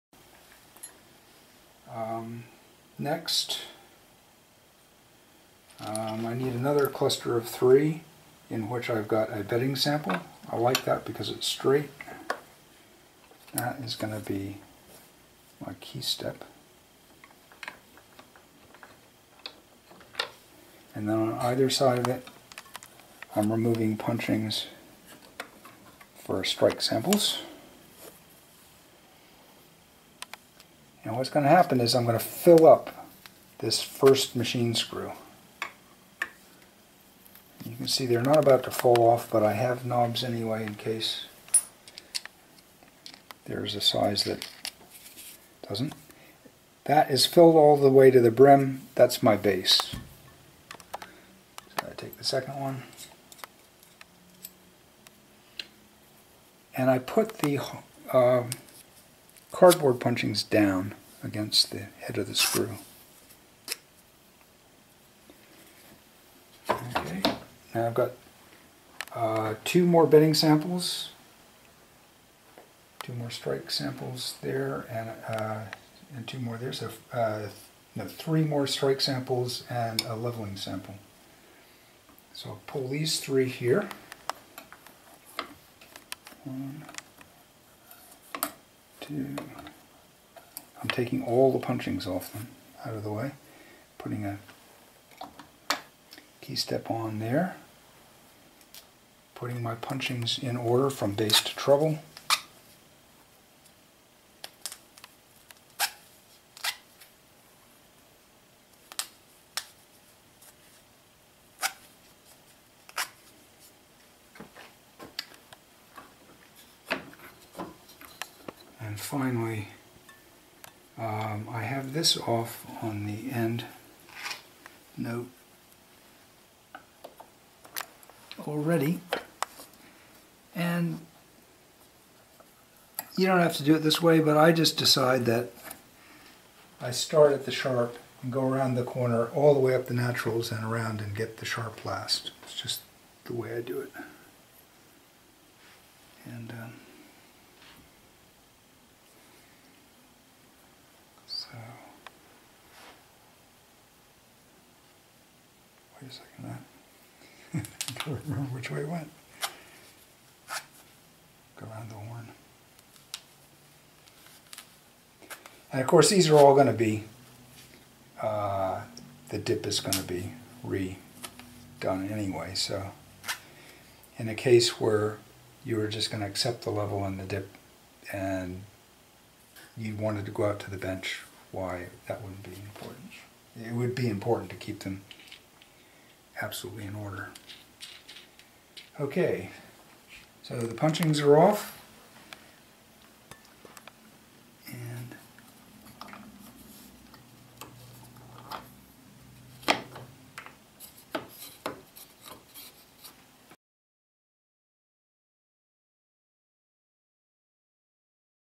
Regulate grand piano – 16 Keysteps on
Video number 16 in a playlist of Chris Brown RPT regulating a Yamaha C3 grand piano using Grandwork protocols and a Grandwork Regulation Station Deluxe. for information on Grandwork tools and training.